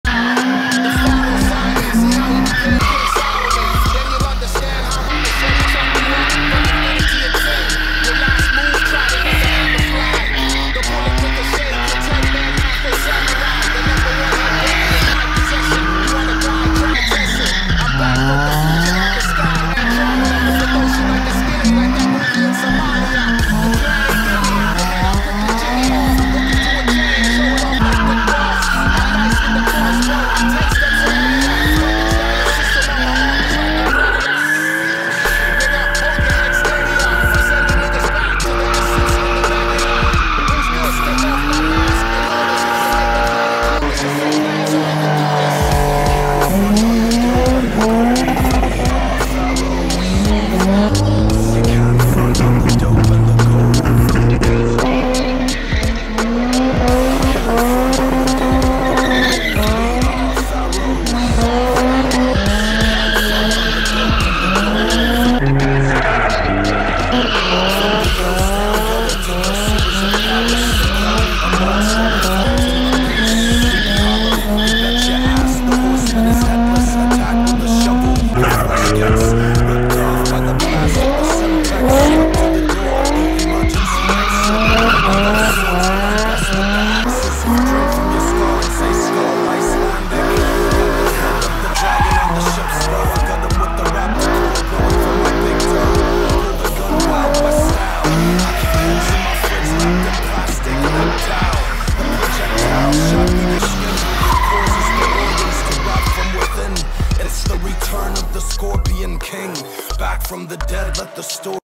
2021 FIRST WEEKEND IN JINO TOYOTA ALTEZZA NISSAN 350Z BMW E34 DODGE INFINITI DRIFT BURNOUT DONUTS
ბარო სასტავ როგორ მოგეწონათ ვიდეო არხის განვითარებითვის დაალიკეთ და გამოიწერეთ გვერდი მეტი მოტივაცია მეტი კონტენტია.